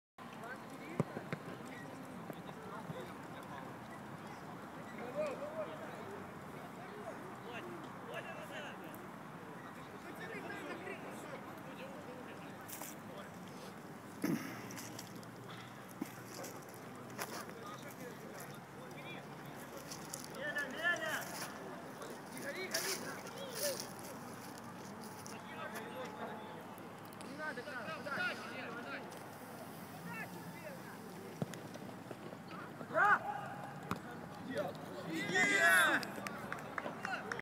Goal for FC Podil Kyiv at 'death match' stadium FC Start
Youth team play Arsenal Kyiv, November 18, 2013